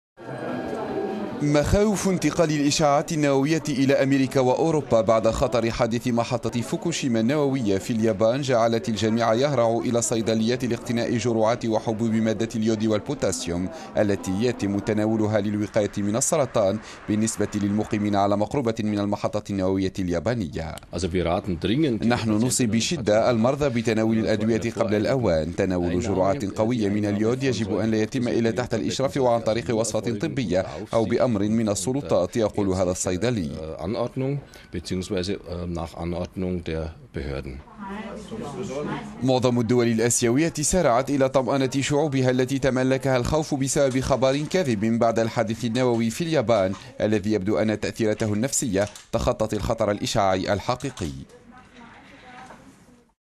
مخاوف في أميركا وأوربا من...
مخاوف إنتقال الإشعاعات النووية إلى أميركا وأوربا بعد خطر حادث محطة فوكوشيما النووية في اليابان، جعلت الجميع يهرع إلى الصيدليات لإقتناء جرعات وحبوب مادة اليود والبوتاسيوم، التي يتم تناولها للوقاية من السرطان بالنسبة للمقيمين بالقرب من المحطة النووية اليابانية.